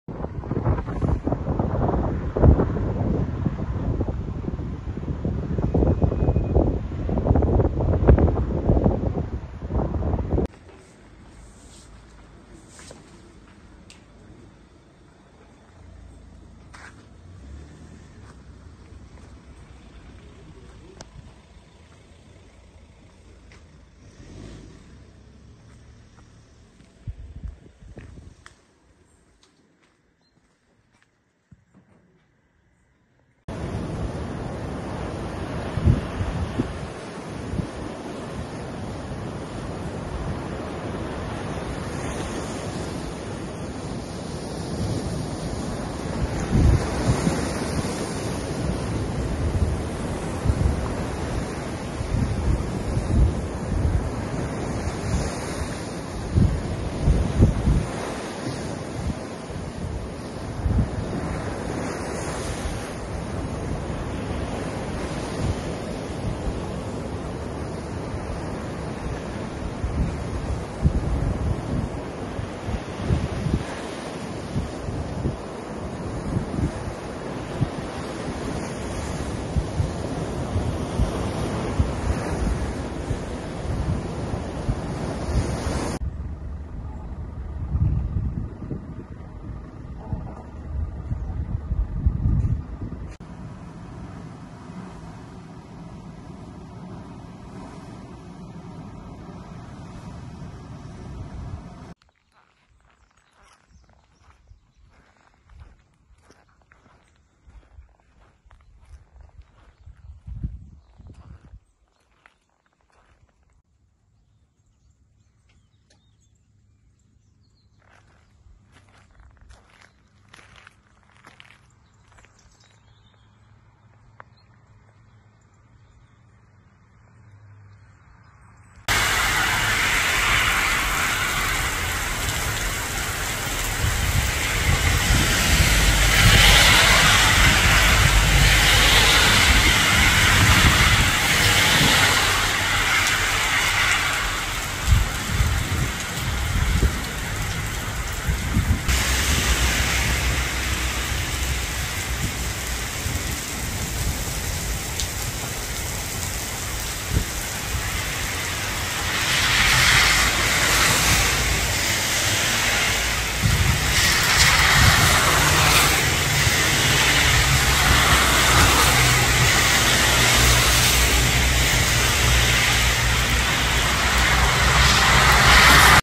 Ζημιές κατά το πρώτο πέρασμα της κακοκαιρίας από τη Δυτική Ελλάδα
Έπεσαν δέντρα, ξηλώθηκαν σκεπές και κατέρρευσαν κολώνες της ΔΕΗ